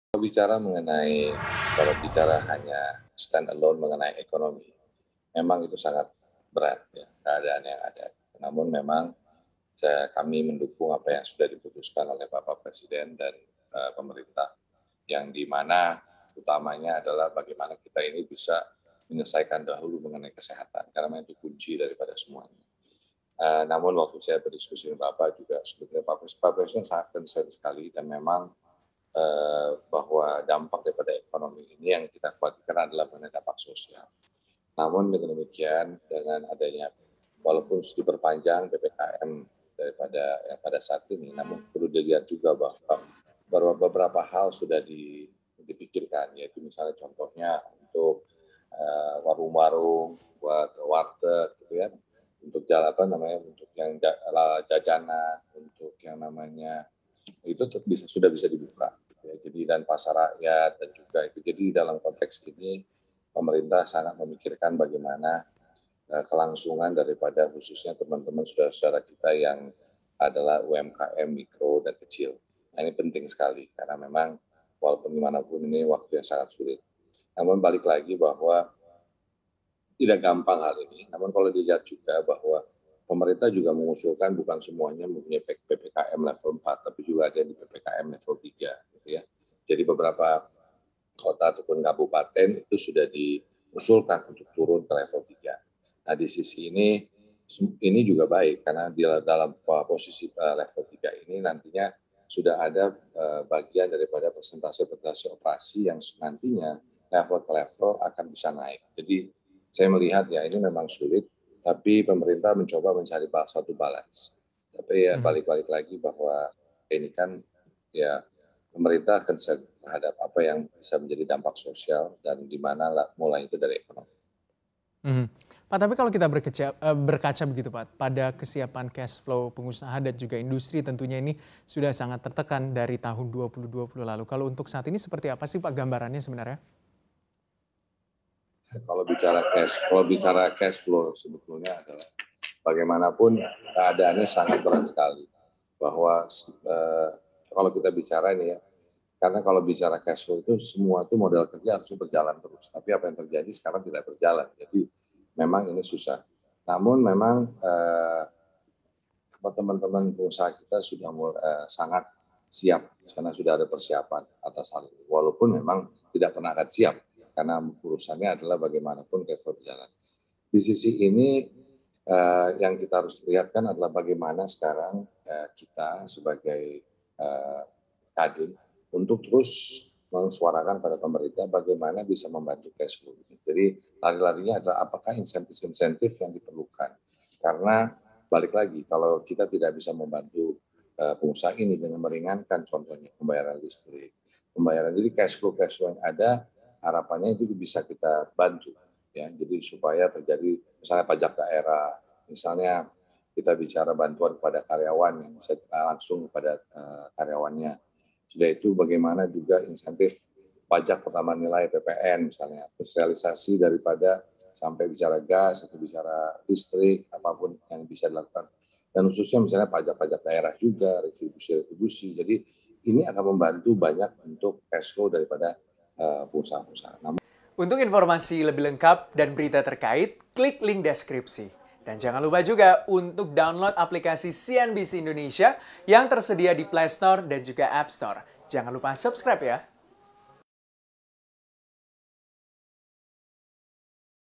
Hadapi PPKM Level 4, Kadin Dorong Insentif ke Dunia Usaha
Berita Lainnya: Pemda Masih Lelet Sebar Bansos, Ancaman Sri Mulyani Dicuekin?

Ketua Umum Kadin Indonesia, Arsjad Rasjid menyebutkan saat ini penanganan pandemi menjadi hal yang utama untuk dilakukan meskipun memberi dampak negatif ke sektor ekonomi. Diharapkan perpanjangan PPKM Level 4 yang memberikan pelonggaran aturan terhadap kelangsungan usaha mikro dan kecil.

Seperti apa Kadin melihat kesiapan pelaku usaha menghadapi aturan PPKM Level 4? Selengkapnya simak dialog Muhamamd Gibran dengan Ketua Umum Kamar Dagang dan Industri (Kadin) Indonesia, Arsjad Rasjid dalam CNBC Indonesia (Senin, 26/07/2021)